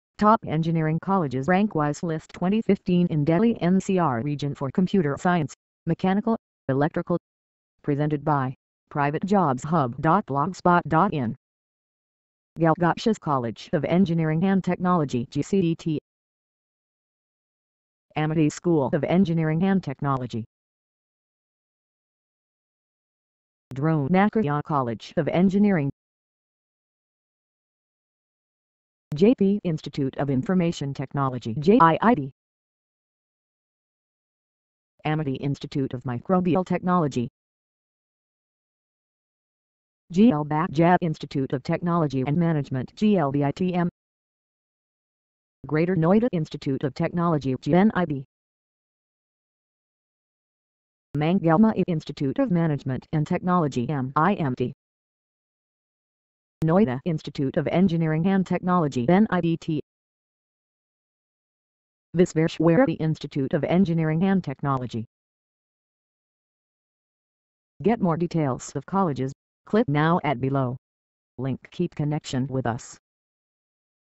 Top Engineering Colleges Rank Wise List 2015 In Delhi NCR Region For Computer Science, Mechanical
The no. of Engineering colleges are increasing day by day with regions of Delhi/NCR among the top.